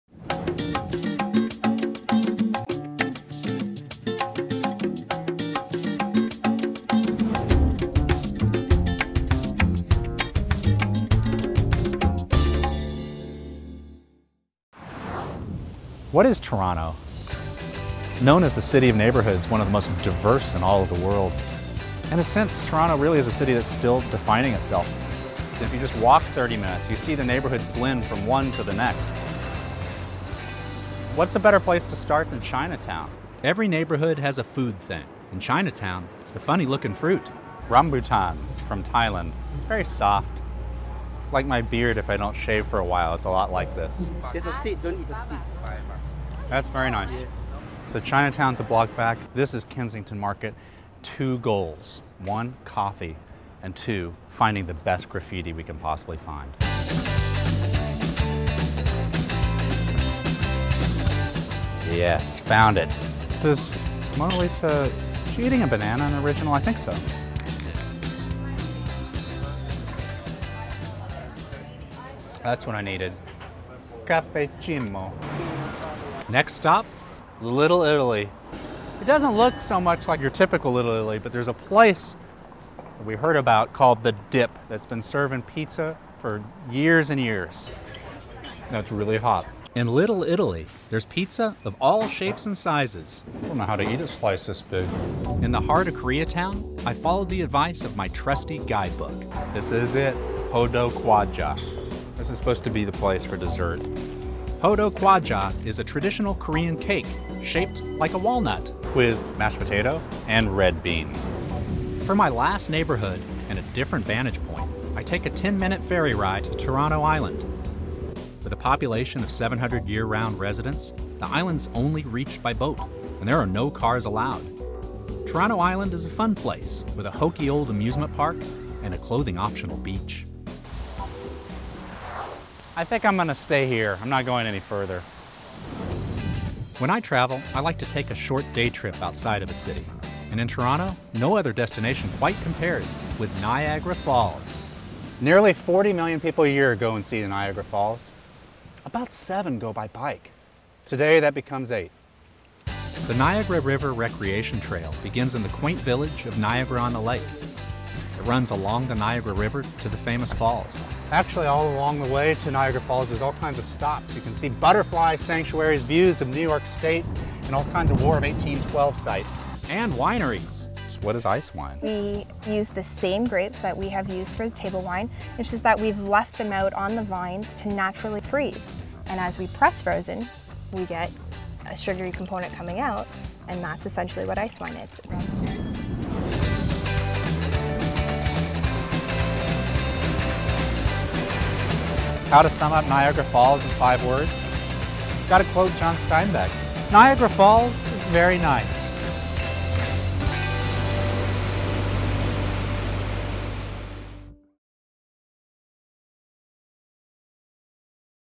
Toronto, Ontario (Canada) - Avec Robert Reid de Lonely Planet
Rencontrez l'éditeur de voyage du Lonely Planet, Robert Reid. Robert a cueilli des baies, éclusé des bières et tracé son chemin à vélo partout au Canada, dans une quête pour découvrir le pays comme un local.

Robert se fraie un chemin culinaire parmi les nombreuses hottes de Toronto, goûte un vin doux canadien et emprunte un vélo pour visiter une certaine cascade.

Contenu vidéo fourni par Lonely Planet: Lonely Planet a réalisé cette vidéo pour la Commission canadienne du tourisme. La Commission canadienne du tourisme n'a eu aucune influence sur son contenu. Les opinions exprimées sont celles de Lonely Planet, exprimées sans crainte ni faveur.